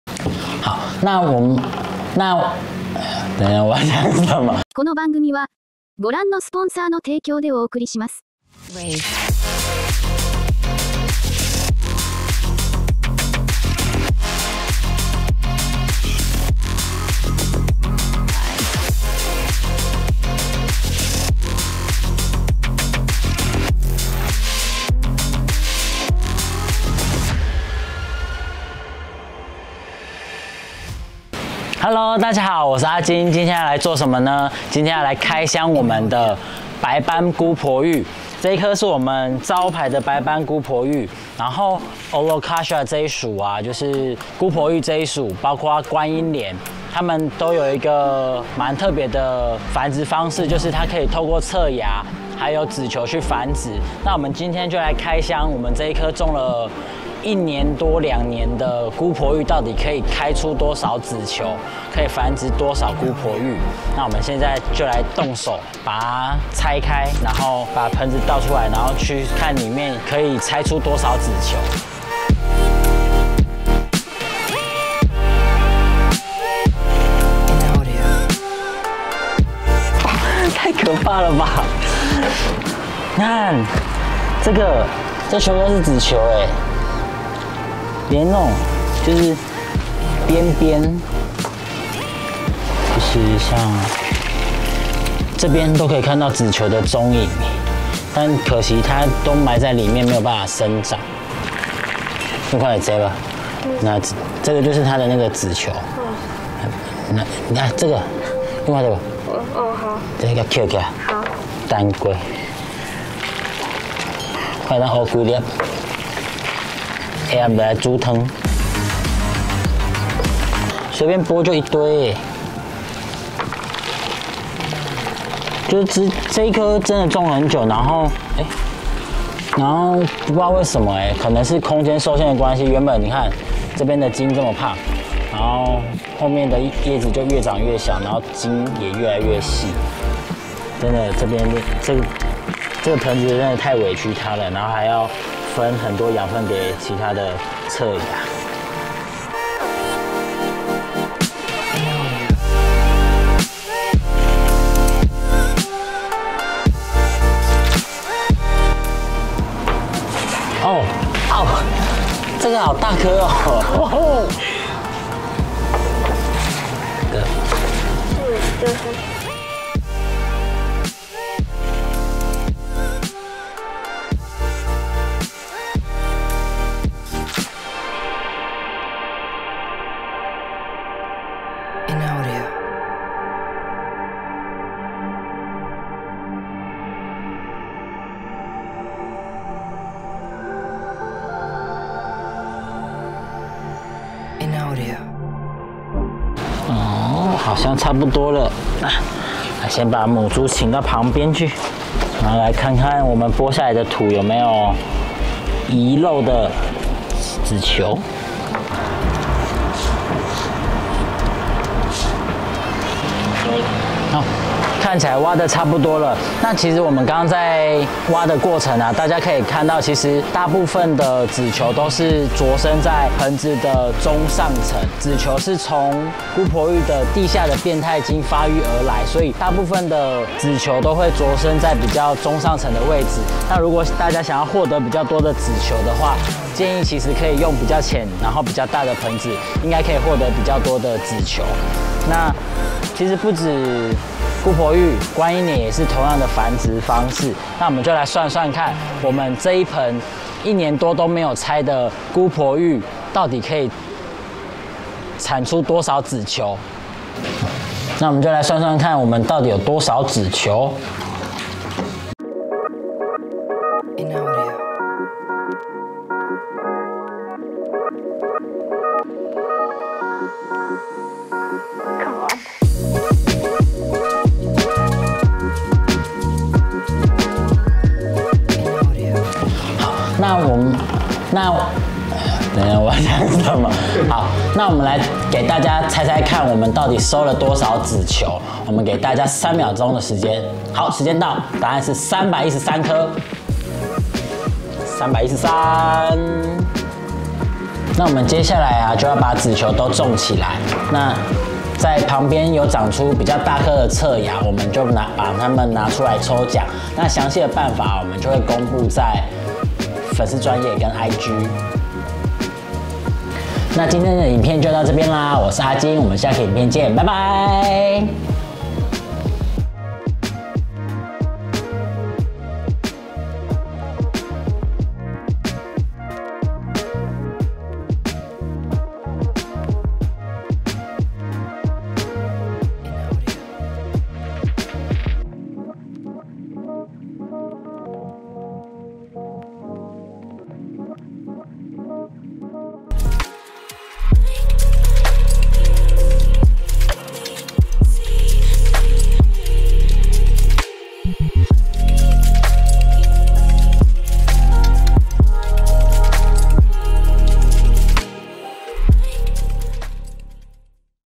白斑姑婆芋老欉居然能產出這麼多子球!!! 姑婆芋 觀音蓮 繁殖方法 拆芽 子球 Alocasia macrorrhiza (L.) Schott & Endl.
空中花園的白斑姑婆芋老欉開箱啦！！！

回頭查了一下白斑姑婆芋是在2020/07🪴購入
2021/10左右換到這個🪣大盆
短短的一年半左右竟然產出這麼多子球🌱!
真是太有成就感了🥰🥰🥰

10:00準時首播！！！

底下任意留言抽白斑姑婆芋
🌱即日起至2022/01/23
🌱只要在底下任意留言
🎁抽白斑姑婆芋1棵
🎁各平台一棵（花園粉專、花園IG及花園YT頻道）
🎰2022/01/26公佈得獎幸運兒🥰